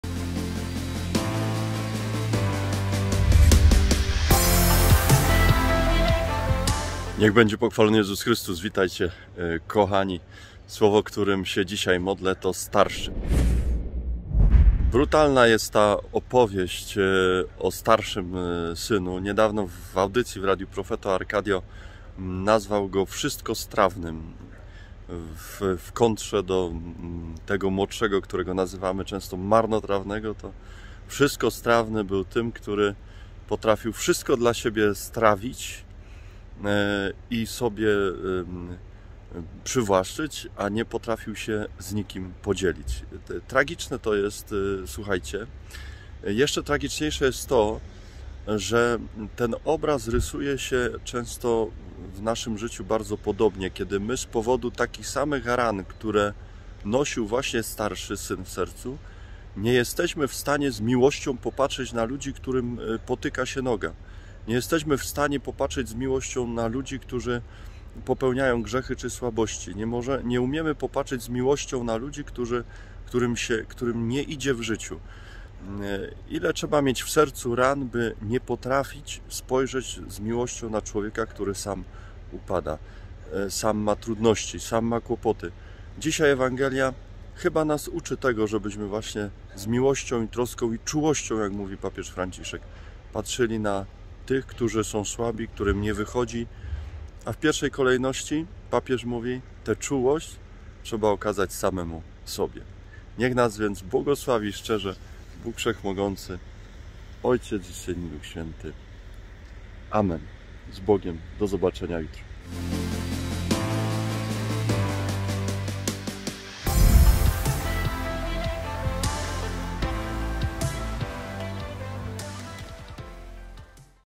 Ks. Michał Olszewski SCJ - VLOG JEDNO SŁOWO - Starszy
"Tymczasem starszy jego syn przebywał na polu. Gdy wracał i był blisko domu, usłyszał muzykę i tańce." Łk 15,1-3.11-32